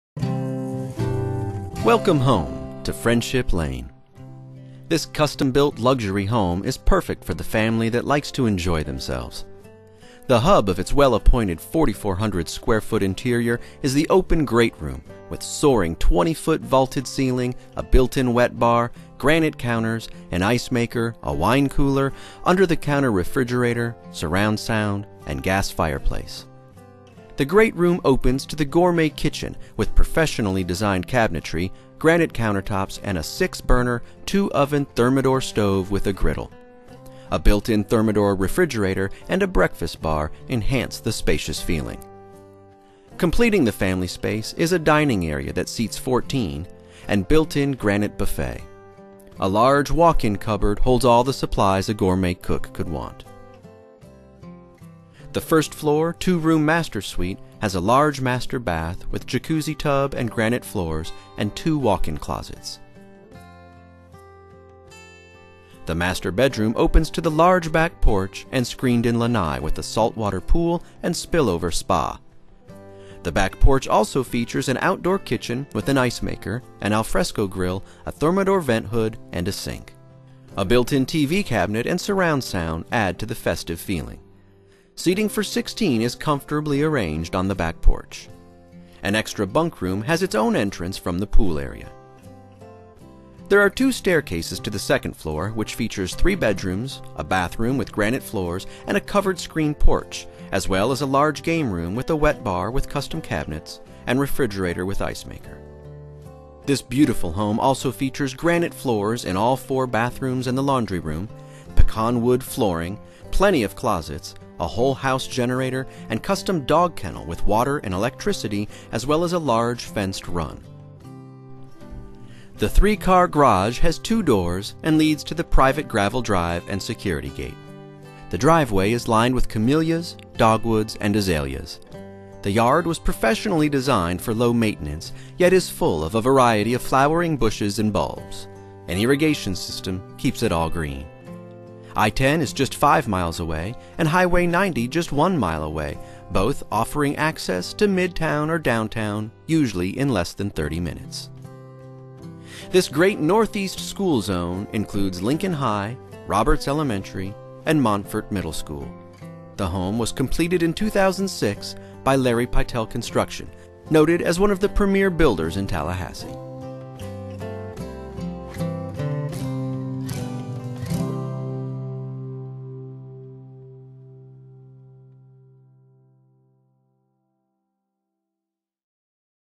Luxury Home For Sale - Friendship Ln, Tallahassee, FL
This custom built luxury home is perfect for the family that likes to enjoy themselves. The hub of its well-appointed 4418 square foot interior is the open great room with soaring 20 foot vaulted ceiling, a built-in wet bar, granite counters, an icemaker, a wine cooler, under the counter refrigerator, surround sound and gas fireplace. The great room opens to the gourmet kitchen with professionally designed cabinetry, granite countertops and a six burner, two-oven Thermador stove with a griddle. A built in Thermador refrigerator and a breakfast bar enhance the spacious feeling. Completing the family space is a dining area that seats 14, and built-in granite buffet. A large walk-in cupboard holds all the supplies a gourmet cook could want. The first-floor two-room master suite has a large master bath with jacuzzi tub and granite floors and two walk-in closets. The master bedroom opens to the large back porch and screened-in lanai with a saltwater pool and spillover spa. The back porch also features an outdoor kitchen with an icemaker, an Alfresco grill, a Thermador vent hood and a sink. A built in TV cabinet and surround sound add to the festive feeling. Seating for 16 is comfortably arranged on the back porch. An extra bunk room has its own entrance from the pool area. There are two staircases to the second floor, which features three bedrooms, a bathroom with granite floors and a covered screened porch, as well as a large game room with a wet bar with custom cabinets and refrigerator with icemaker. The back stairs from the game room descend to the rear of the kitchen, providing direct access to the garage and the guest/pool bathroom with a roomy walk-in shower and granite floors. Other features are granite floors in all four bathrooms and laundry room, beautiful pecan wood in the great room, kitchen, front stairs and halls, plenty of closets, a whole-house generator, and custom dog kennel with water and electricity as well as a large fenced run. The three-car garage has two doors and leads to the private gravel drive and security gate. The driveway is lined with camellias, dogwoods and azaleas. The yard was professionally designed for low maintenance, yet is full of a variety of flowering bushes, bulbs and vines including over 30 climbing roses on the privacy fence. An irrigation system keeps it all green. 1-10 is just five miles away. A scenic uncomplicated drive on Mahan Drive of 33 minutes brings you to the steps of the Capitol. Custom-made furnishings selected by our designer for the home are available for sale as well. This great NE school zone includes Lincoln High, Roberts Elementary, and Montford Middle School. The home was completed in 2006 by Larry Pytel Construction - noted as one of the premier builders in Tallahassee.